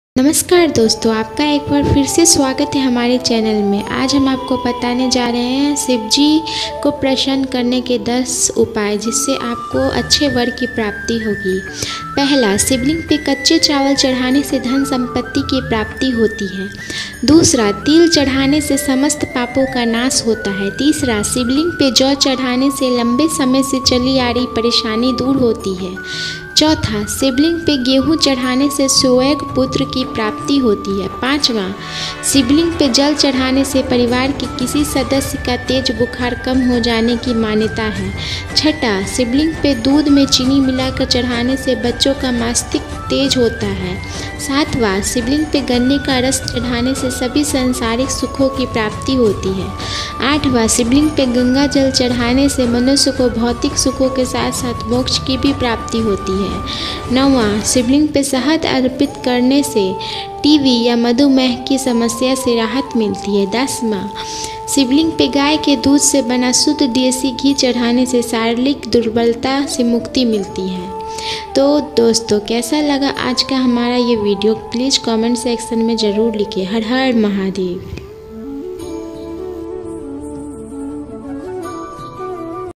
शिव जी को प्रसन्न करने के लिए 10 उपय | शिव जी से वर प्राप्ति के लिए
Mahadev bhakti song
Mahadev bhajan
Shivji bhakti song